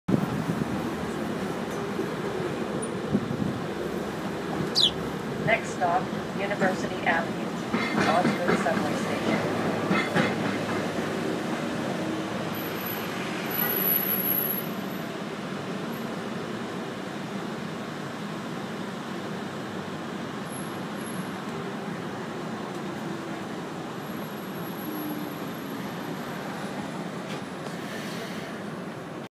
Toronto Rush Hour Hitchhiker
This little sparrow road the 501 Queen street car this morning.